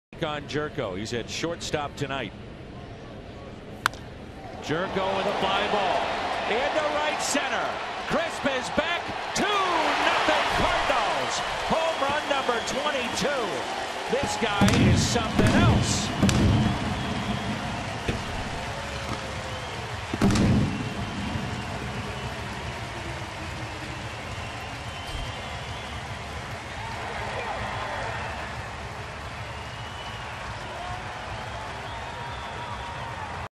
OAK@STL: Gyorko drills a two-run homer to center
Jedd Gyorko puts the Cardinals on the board with a two-run home run to center field in the bottom of the 1st